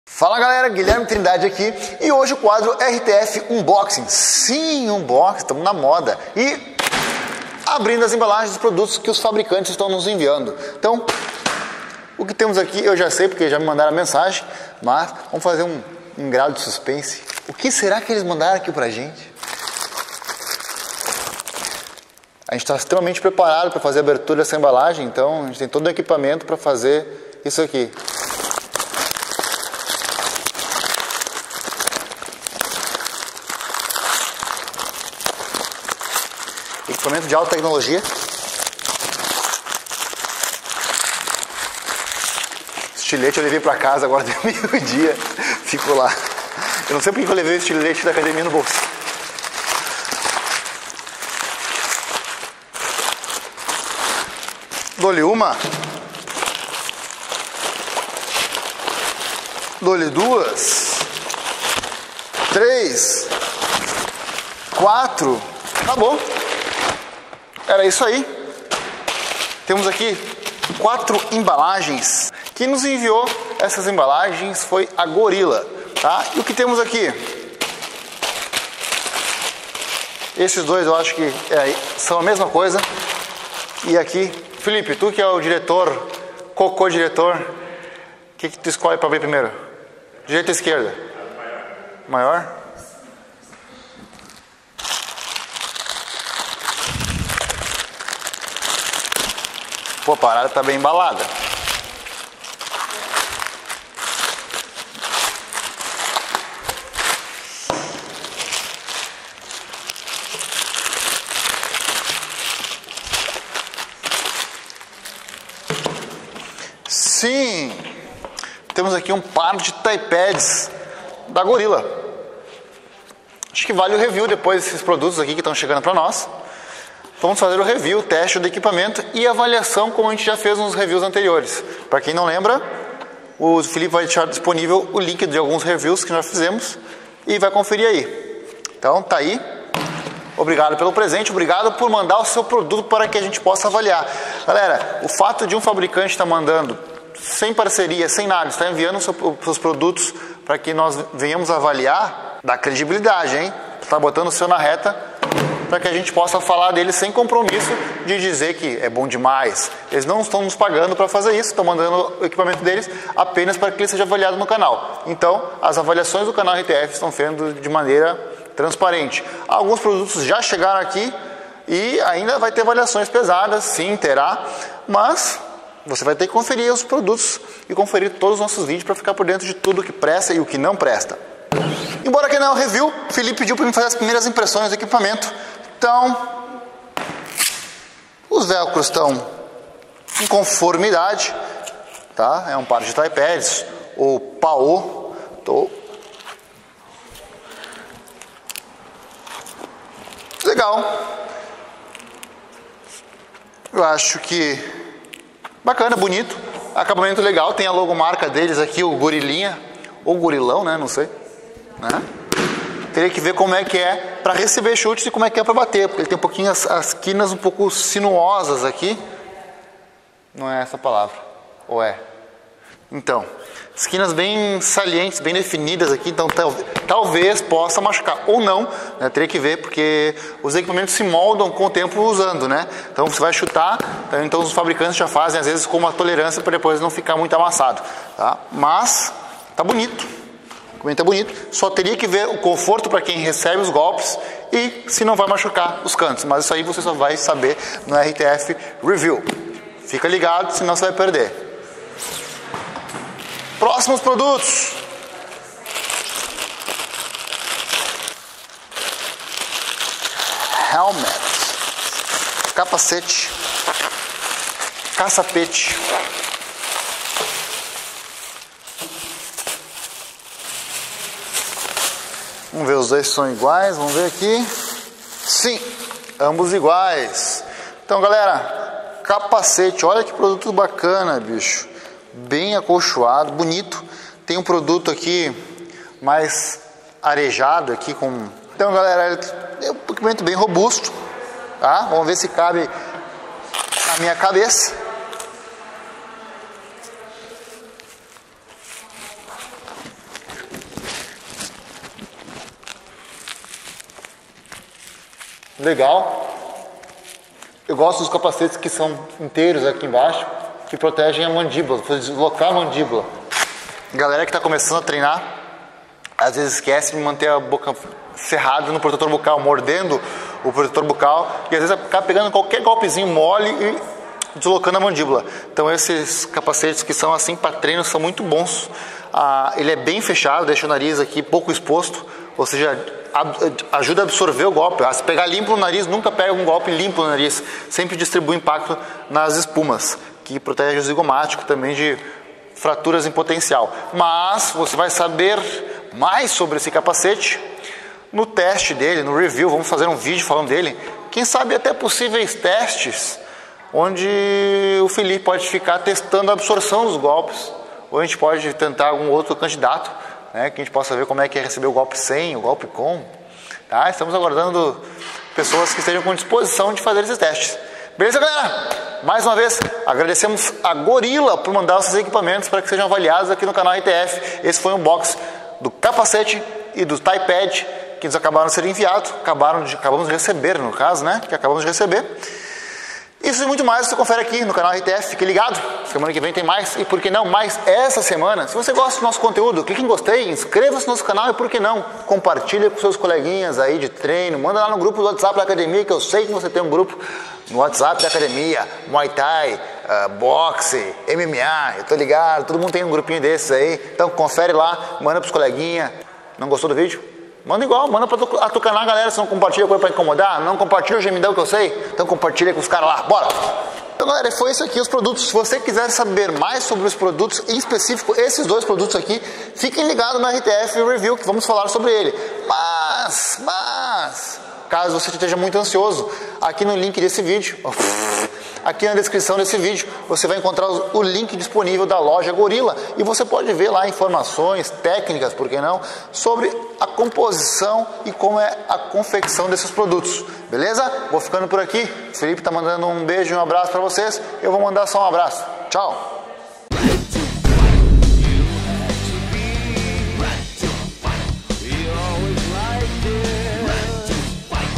Capacete e Thai Pads Gorilla - Unboxing
A marca Gorilla Fight nos mandou alguns produtos para que possamos dar nossa opnião. Confira nesta vídeo o unboxing e primeira impressões desses produtos.